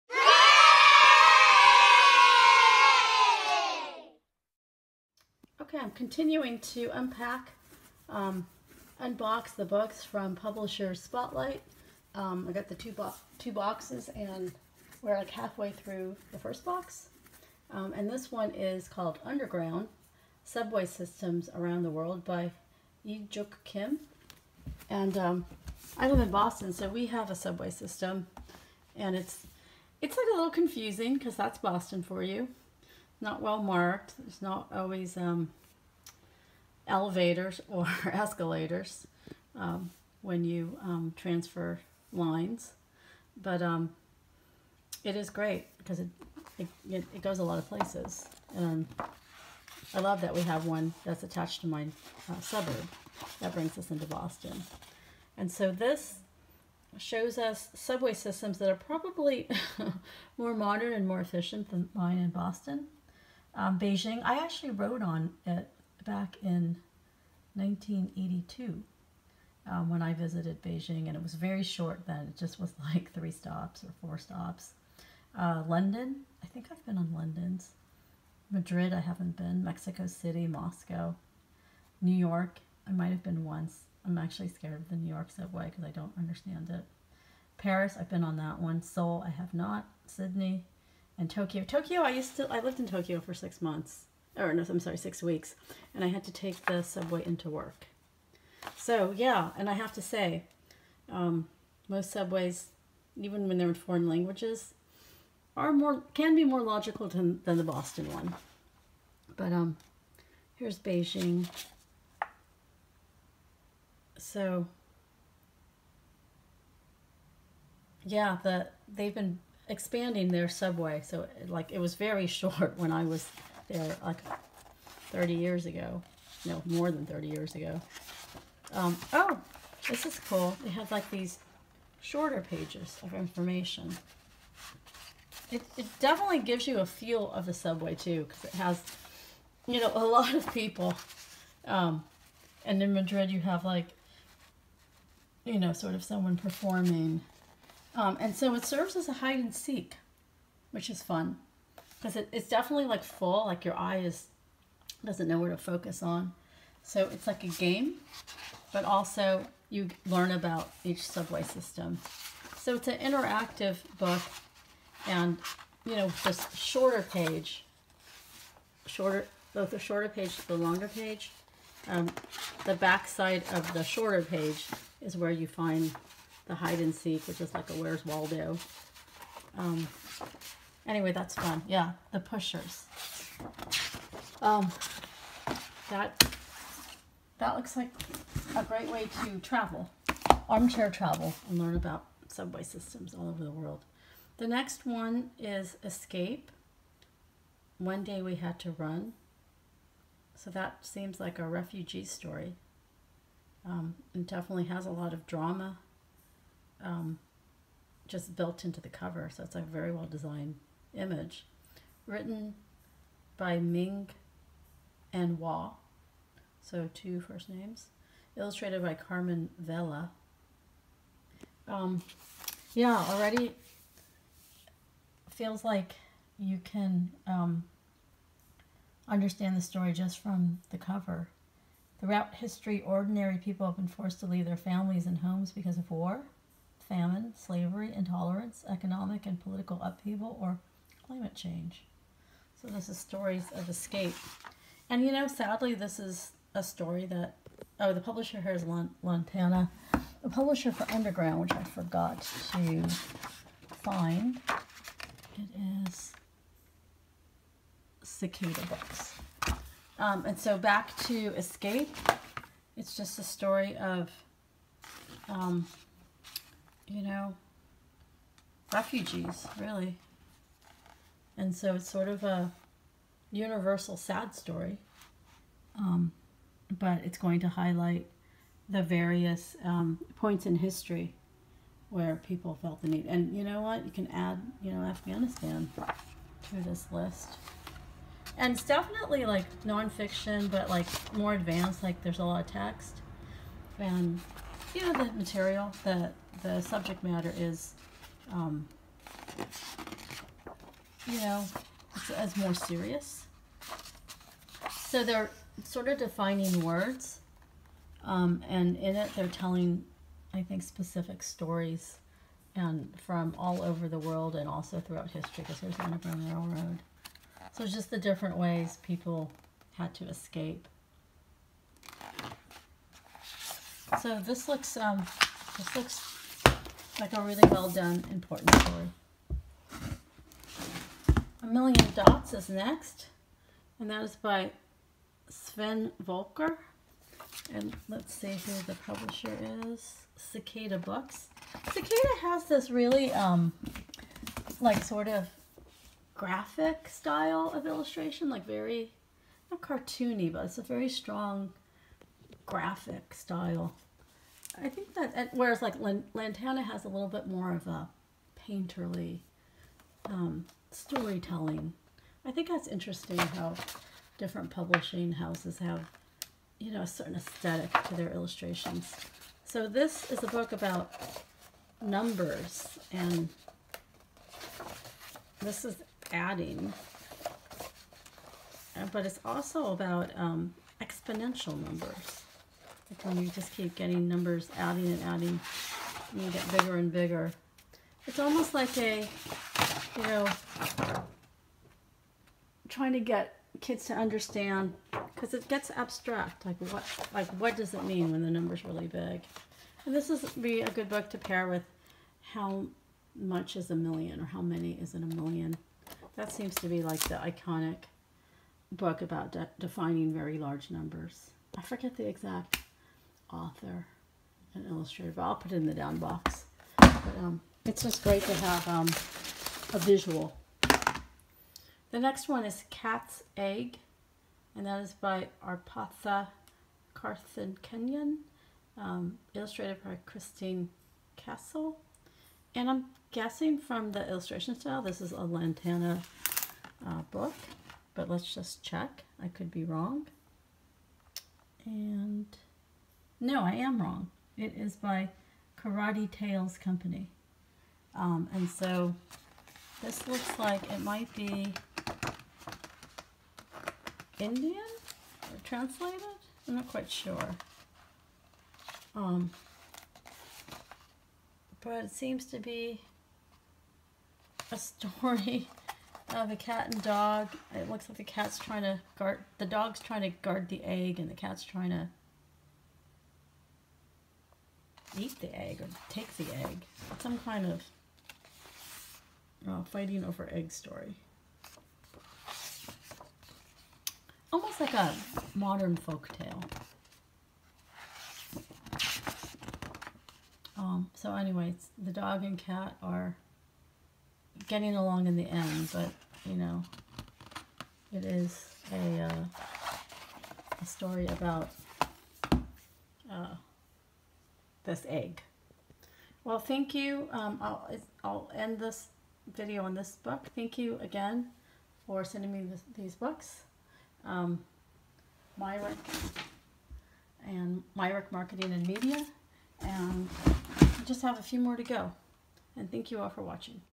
Unboxing//4 Picture Books (2021)
- Underground: Subway Systems Around the World (a seek and find book) by Uijung Kim, published by Cicada Books
- Cat's Egg by Aparna Karthikeyan, illustrated by Christine Kastl, published by Karadi Tales Company
- Escape: One Day We Had to Run by Ming & Wah, illustrated by Carmen Vela, published by Lantana
- A Million Dots by Sven Volker, published by Cicada Books

I have a blog post on Modern Immigration & The Refugee Experience Books for Kids:

I have math picture books that include large numbers:
Our 10 Favorite Math Picture Books

I mentioned How Much Is a Million? by David M. Schwartz, illustrated by Steven Kellogg